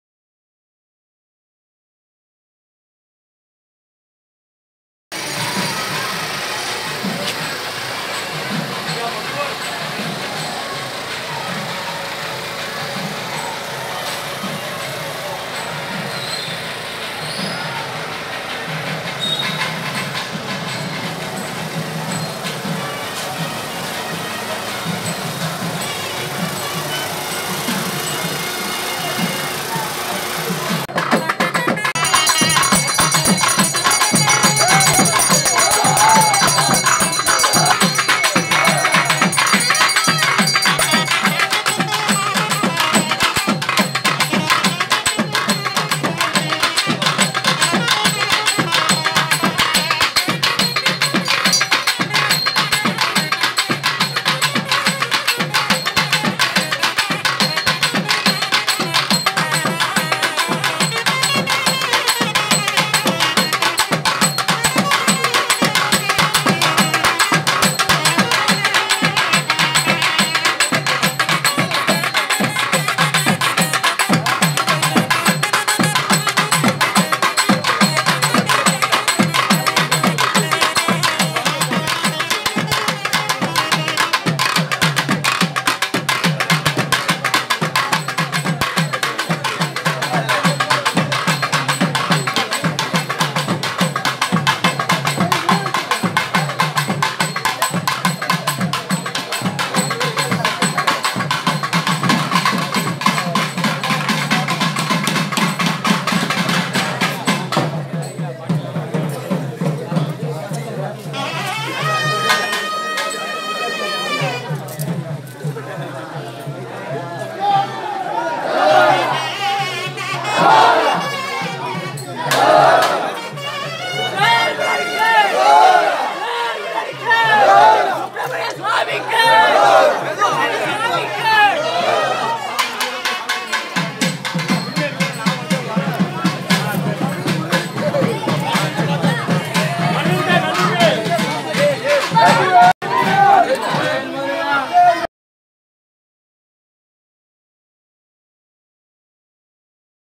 அய்யா கோவில் சப்பரம் ஸ்ரீமன் நாராயண சுவாமி மாசி திருவிழா அடையகருங்குளம் Village Temple Festival
அடையகருங்குளம் ஸ்ரீமன் நாராயண சுவாமி  திருக்கோயில் 8-ம் நாள் மாசி திருவிழா 2021

Sriman Narayana Swamy Temple
Adayakarunkulam
Tirunelveli District

Ayya temple - Village temple festival celebration